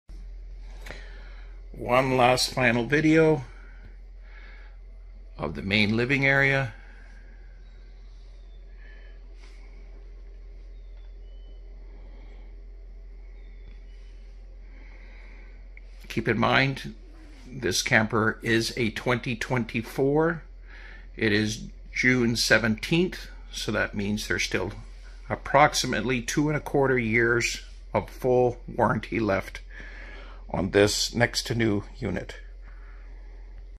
2024 GRAND DESIGN SOLITUDE 391DL For Sale
Grand Design Solitude Fifth Wheel 319 DL

Like new, hardly used. Beautiful, immaculate, pristine condition!
Two bedrooms, two bathrooms.
Dual entry.
Five slides.
Kitchen island.
Abundant Storage Inside and Out.
Fireplace.

Two private bedrooms plus and overhead loft above the rear bedroom. King bed in the master bedroom, queen bed in second bedroom. Both have upgraded RV mattresses.
High ceilings, large scenic windows, and an interior width of 8'5".
Five slides whih expand the living space. The upscale kitchen features an isleand with a large sink, a 20 cu. ft. refridgerator and a 24" residential gas oven, large microwave, pantry and plenty of cabinet space. Also includes a free standing dinette.
The second bedroom features its own private bath and entry. Both bathrooms feature a shower with a skylight, vanity, sink and toilet.
Ample storage with plenty of closet space, cabinets and drawers.
Three smart tv's included with state of the art electronics.
Soft Start air conditioning units added to enable use with 30 Amp service.
The camper is currently on a seasonal sight. There is a 5 year waiting list. As an option the lot can be transferred to the new owner, additional fees will apply.
Large new deck can be negotiated with the trailer as well.

10% Buyers Fee will be added to the selling price.
Call/Text Barry at 431_244_7972
We Can Help With Financing, and Arrange Shipping!!